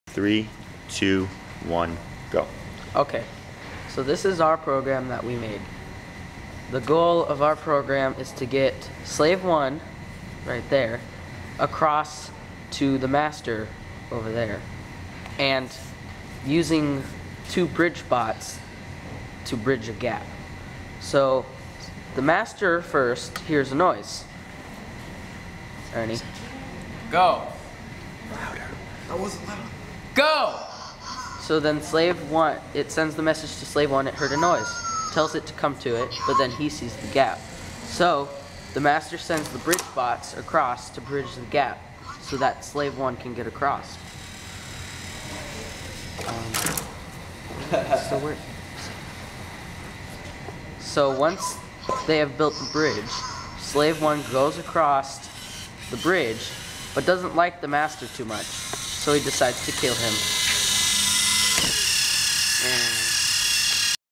BridgeBot.MOV
One robot drives forwards and senses a large drop.  It tells the master, who then tells the two bridge bots to span the gap.  When they are finished, they tell the master, who then tells the first bot it's safe to cross.  The first bot is very rude and decides to smash the master after crossing the bridge.